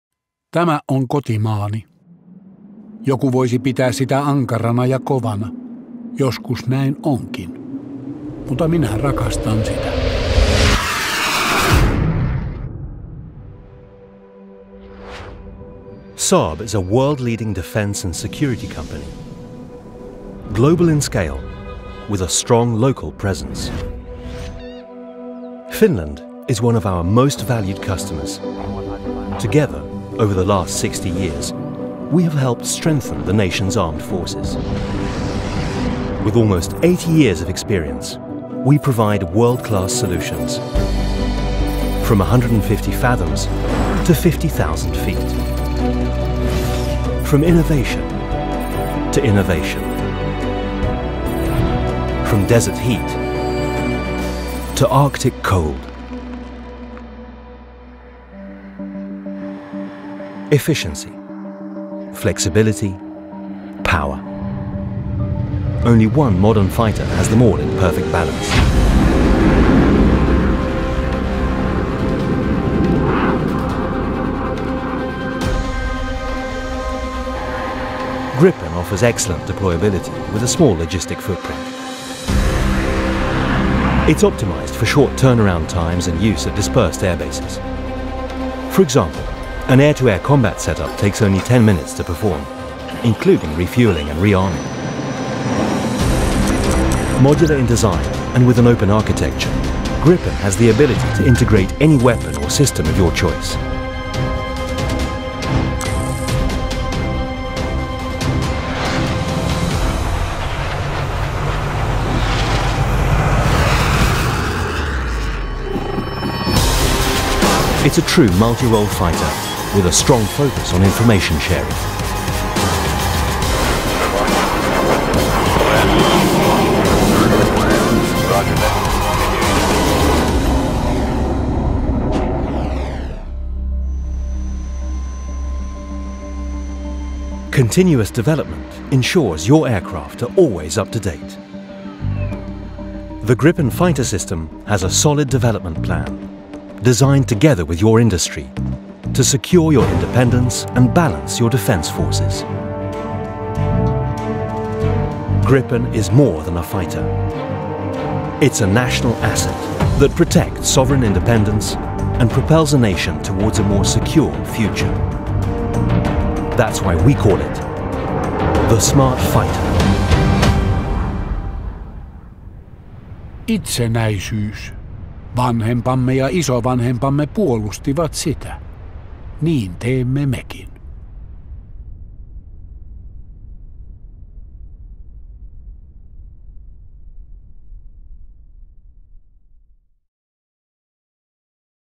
Gripen para a Finlândia, a combinação perfeita
Apresentação da nossa longa cooperação com a Finlândia e sobre as capacidades Gripen na substituição HX F / A-18 Hornet.
Introdução e Outro é em finlandês.